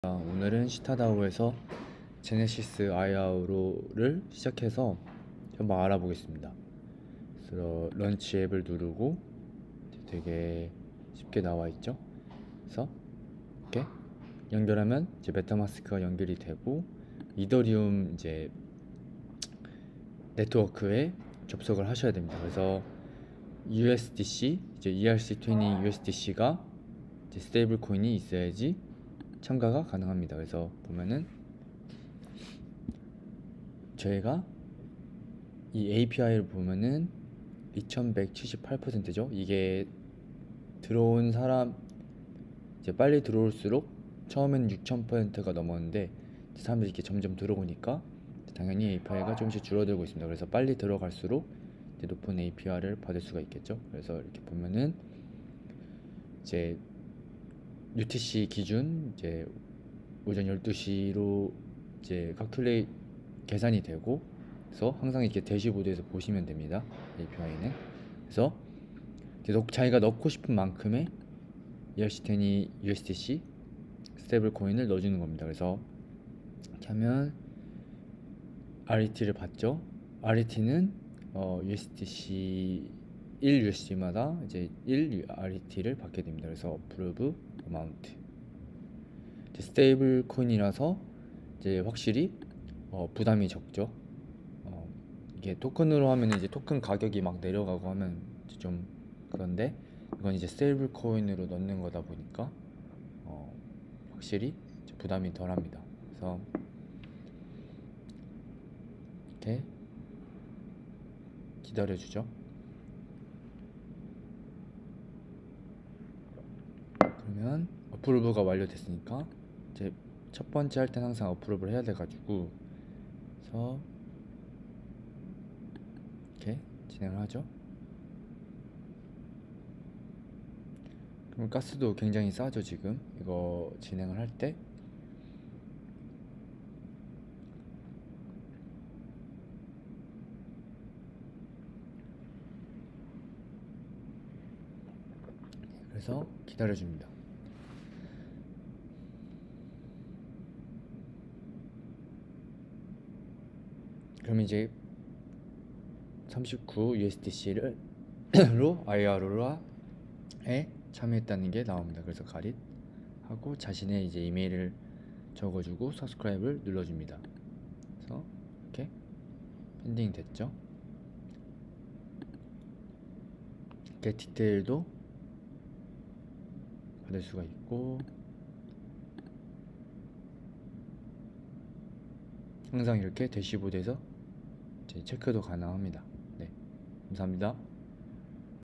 Citadao 시타다오 최초 IRO를 스테이블 코인 USDC를 이용해 최대 6894% APY 받아보자!
IRO 종료일 전에 Softcap이 달성되면 IRO가 성공합니다. Softcap을 달성 못하면 지불한 USDC는 회수가 가능하지만 보상이나 부동산 토큰은 받지 못합니다.

ERC20 UDSC 주소: 0xa0b86991c6218b36c1d19d4a2e9eb0ce3606eb48

주소: 56 Queen Street, Cardiff, CF10 2PX, UK
IRO 기간: 10일
시작일: 2022년 3월 8일 오전 9시 KST

Dune Analytics에서 IRO의 진행 상황을 추적할 수 있습니다.